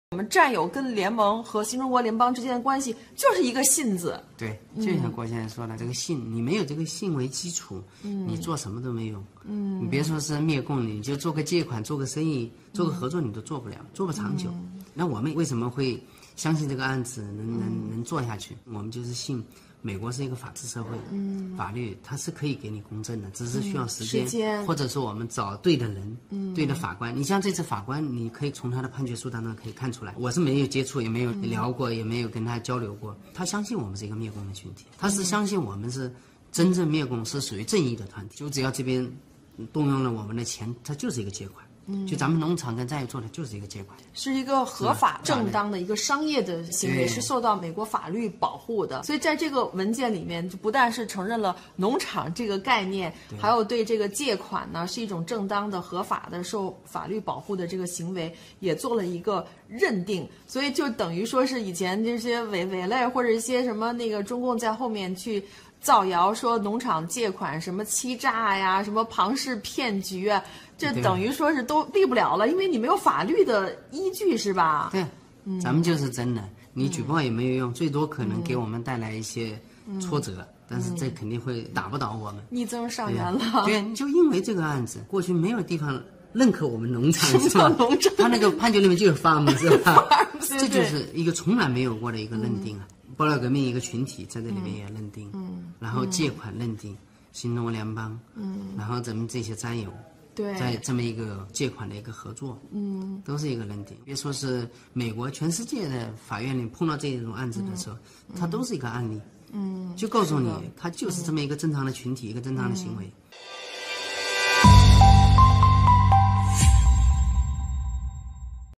长岛哥：法官对九指妖一案的判决不但让伪类们的造谣诬陷不攻自破，还从法律角度上承认了喜马拉雅农场、爆料革命和新中国联邦的正义与合法性！
大家可以订阅以下平台的频道更全面的了解真相
(需先安装discord；捐款自愿，没有法治基金捐款可以加入农场)